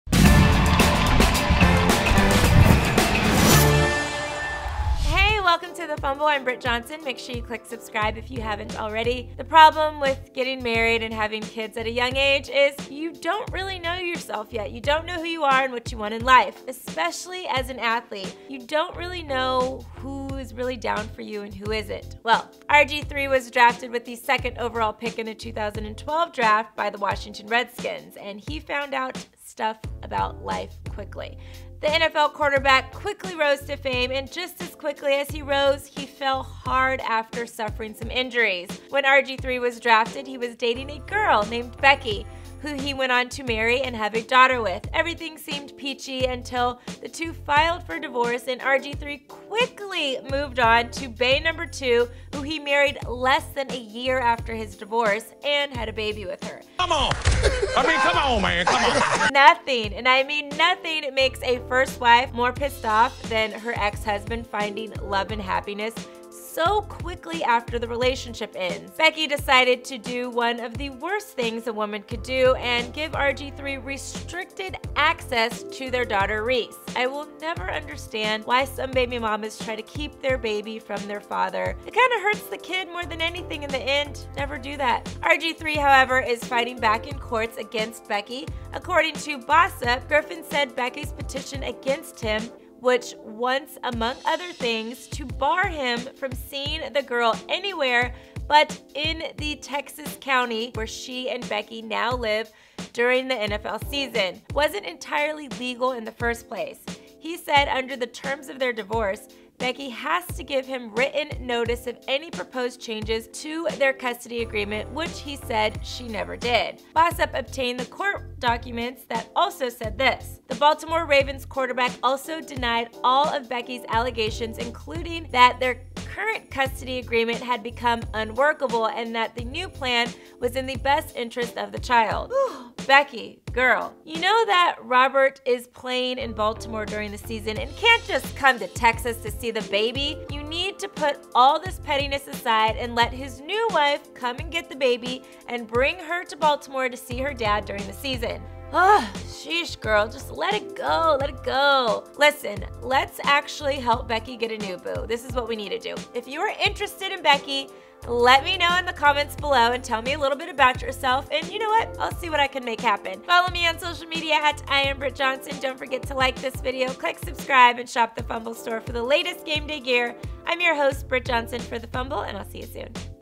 RG3 Goes OFF On Ex Wife For RESTRICTING Access To Kids Because She's BIG MAD He Married Mistress!
The problem with getting married and having kids at a young age is your don't really know who you are and what you really want in life. RG3 is facing that as he battles his ex-wife in court for limiting his access to his kids. The ex-wife is pissed that he moved on so quickly after their breakup. Do you think this ex is just petty?